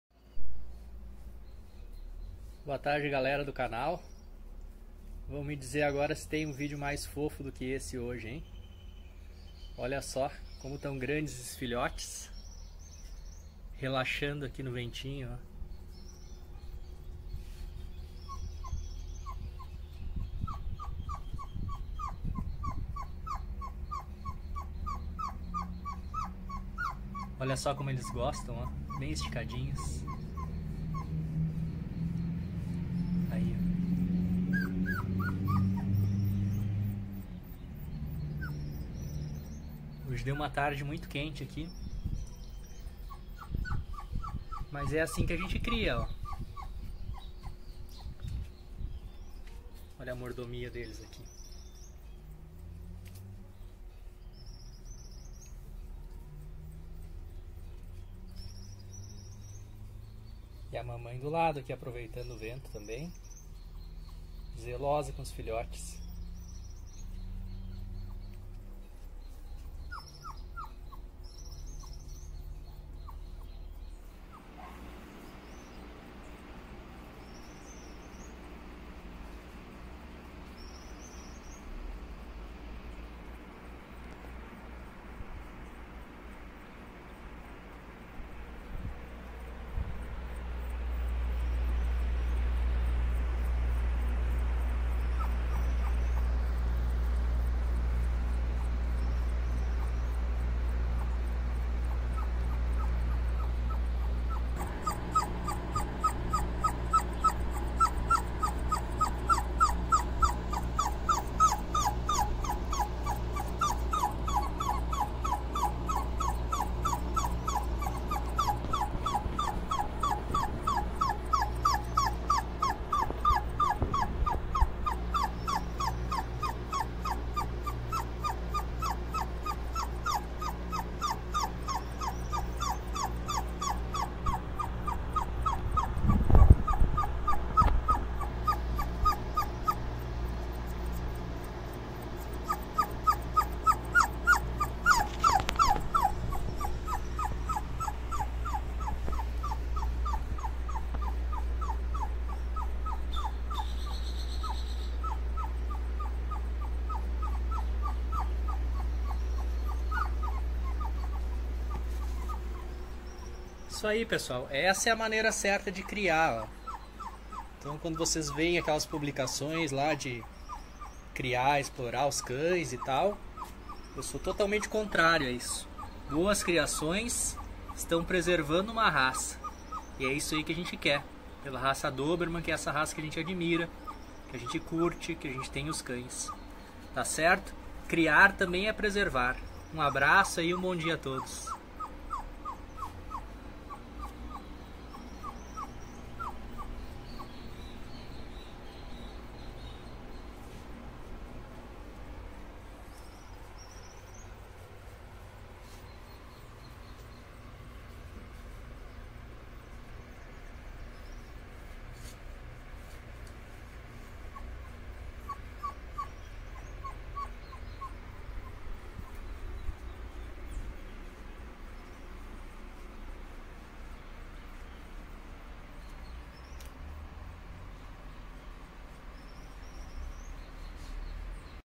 Vídeo mais fofo do dia! Filhotes de Dobermann se refrescando...
Amor, respeito e cuidado com os animais! Paixão por Dobermann´s...